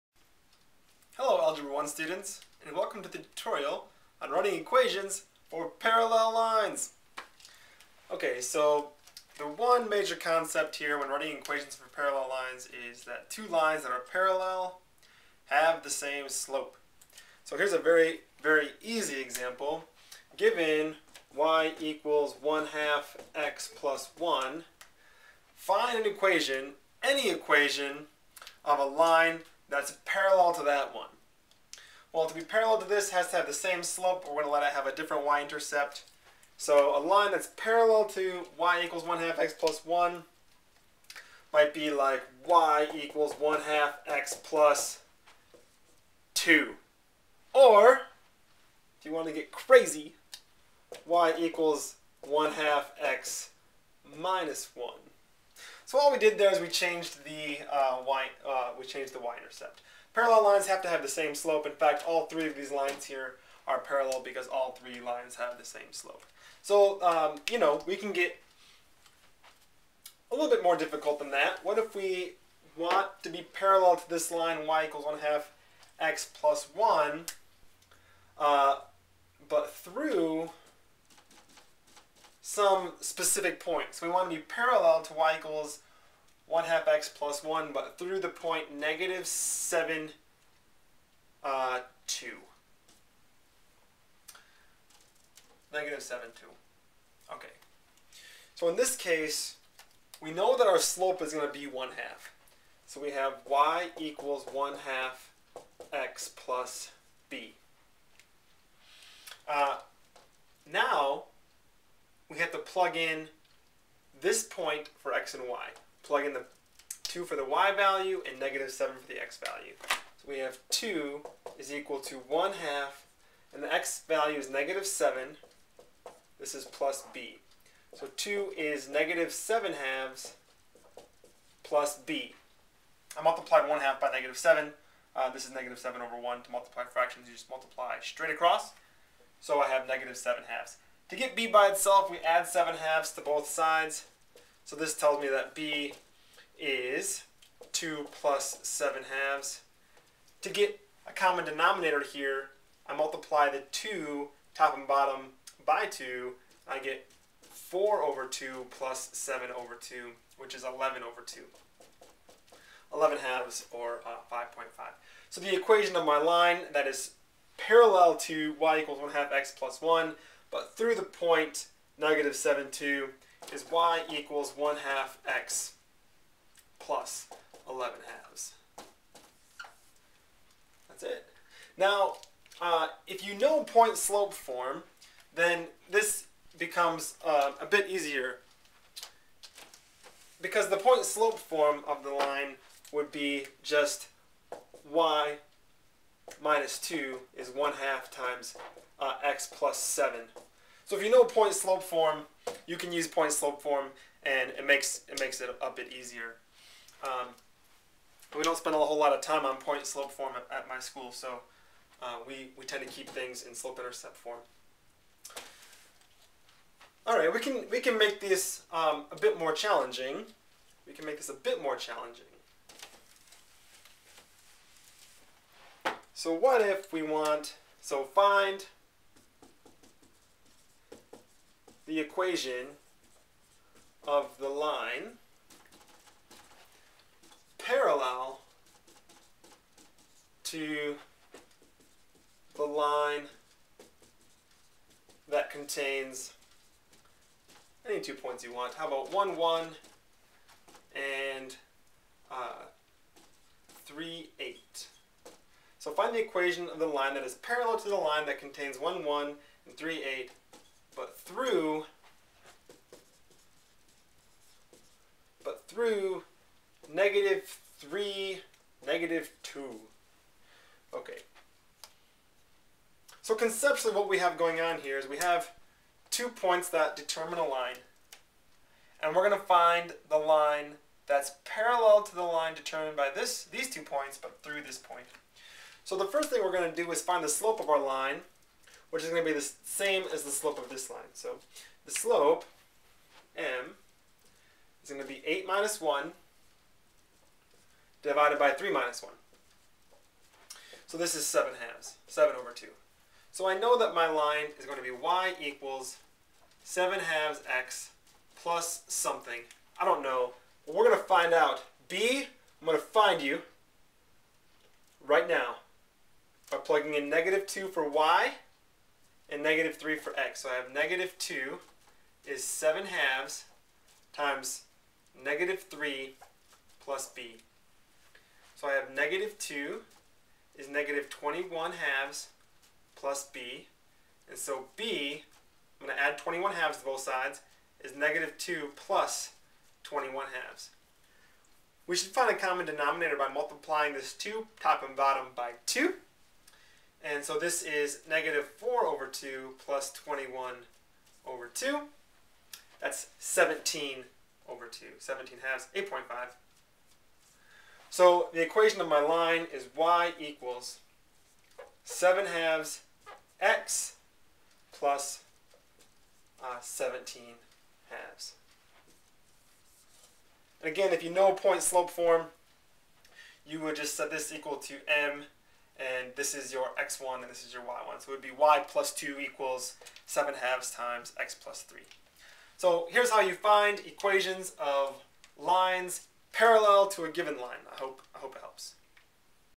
Equations of parallel lines
A few examples of how to write equations of lines parallel to a given line.  Here the focus is writing in slope-intercept form, but point-slope form is a bit less strenuous.

Parallel lines have the same slope ... and that is the name of the game.